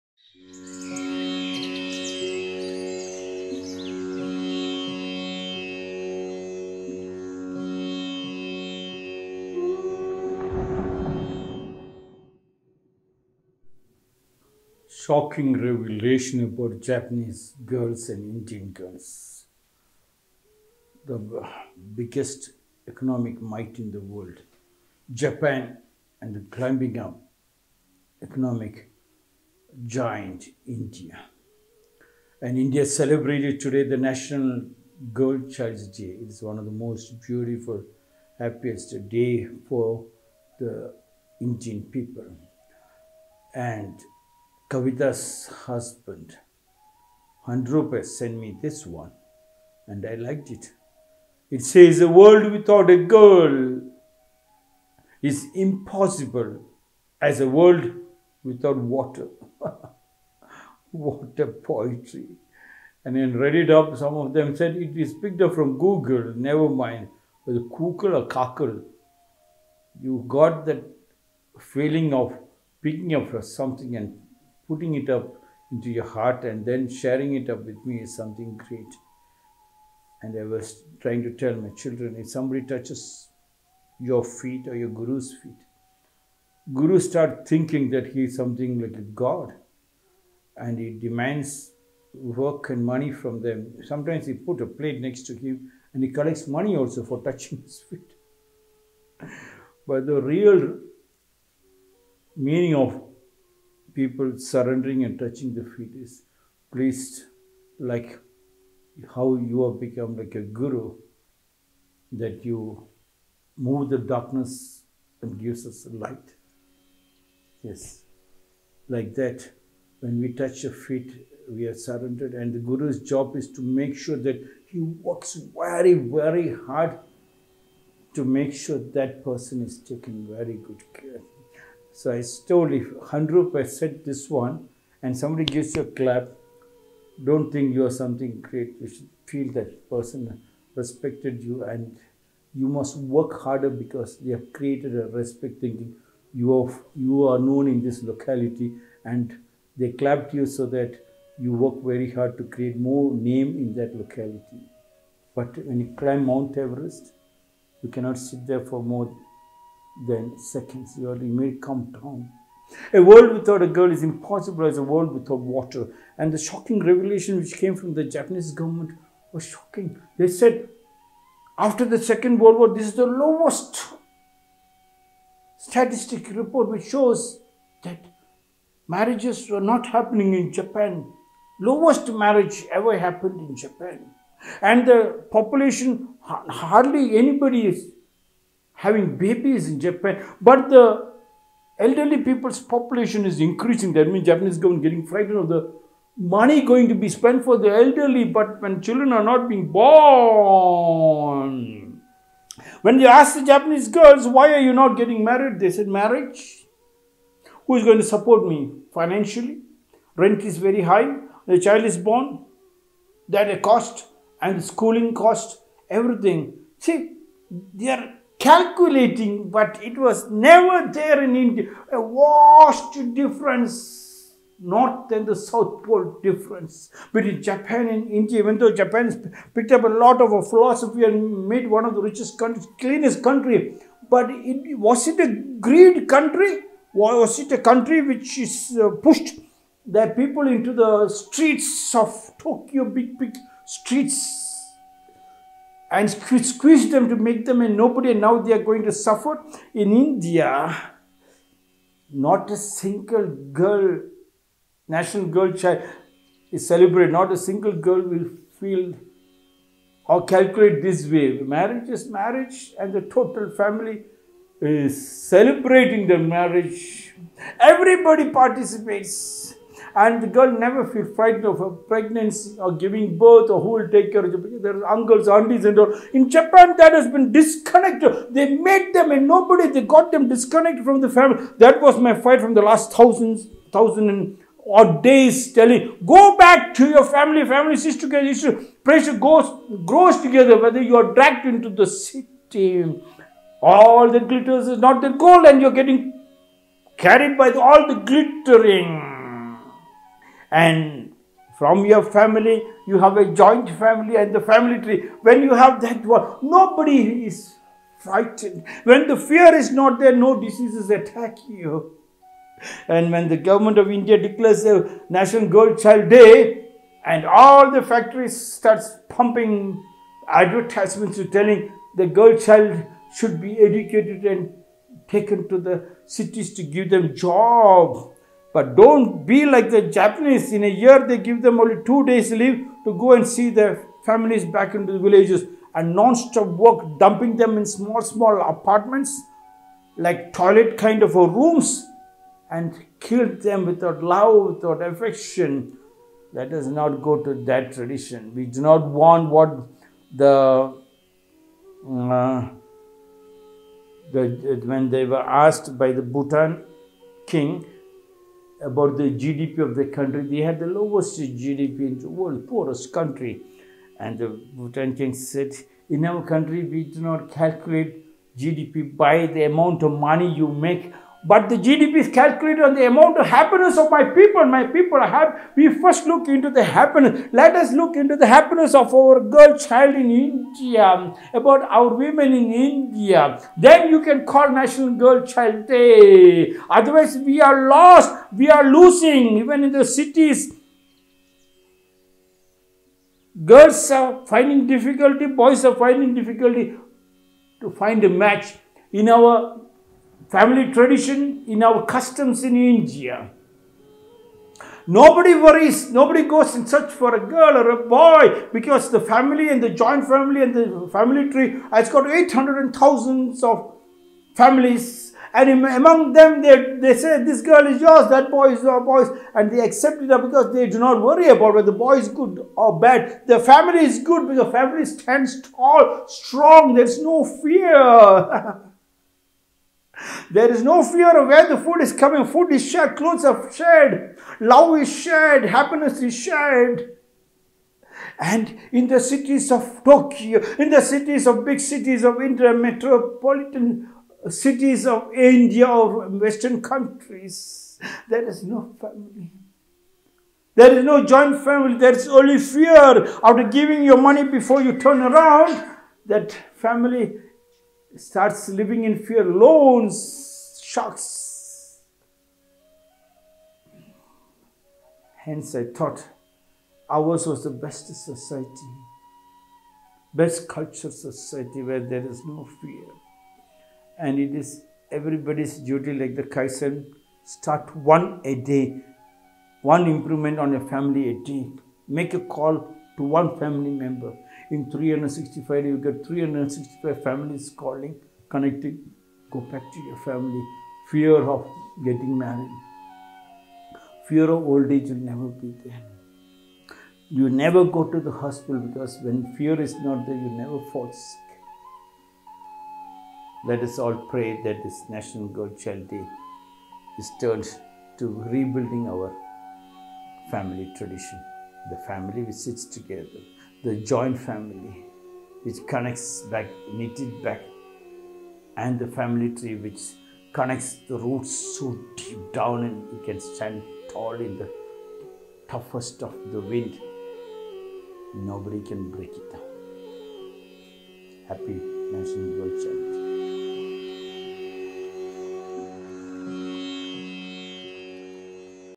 Swami Rakumjee, 1019th short discourse on National Girl Child Day
National Girl Child Day

"A world without a girl is impossible as a world without water... "